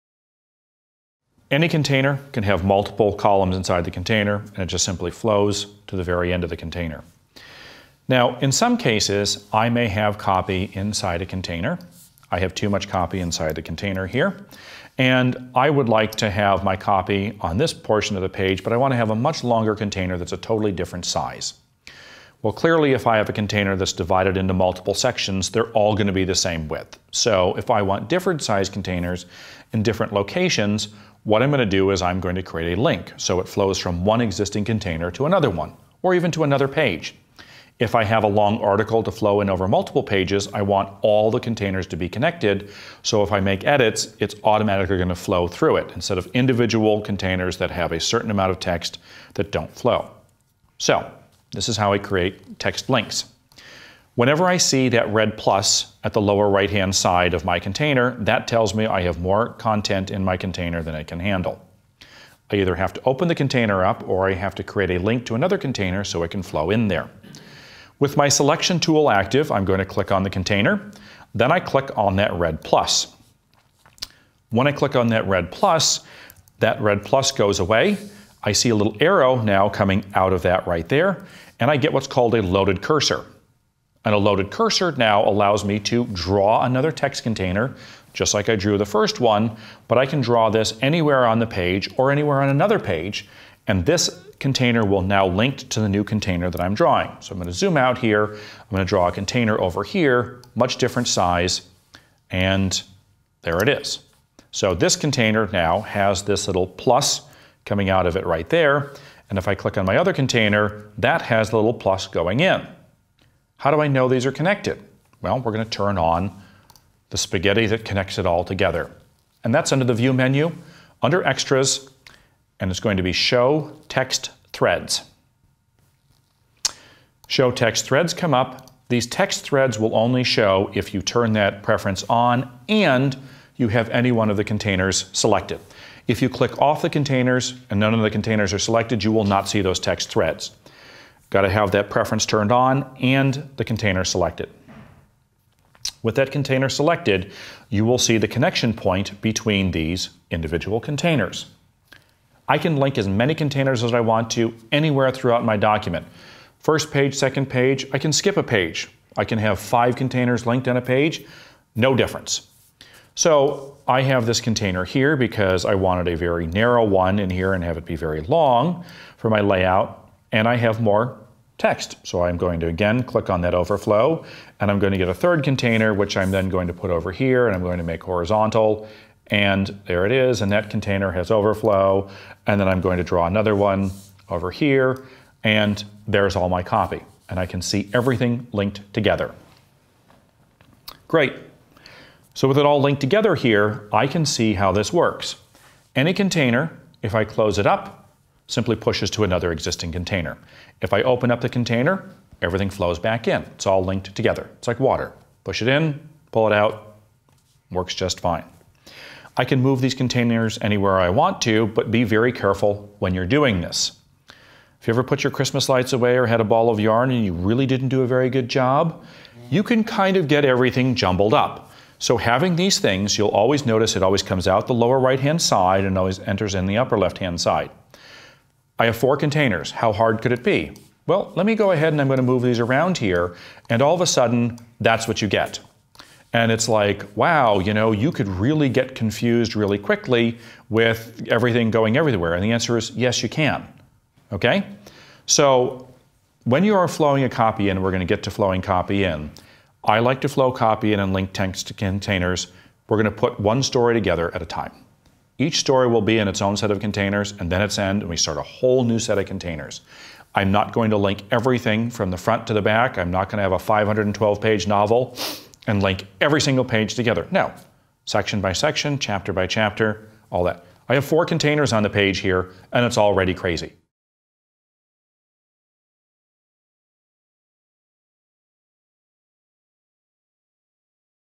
How to Link Text in InDesign
Fitting important text into a design can be a challenge. There a are a couple of options for accommodating your text when working in InDesign. In this clip Jason Hoppe shows you how open up a text container to fit additional text or link text between containers.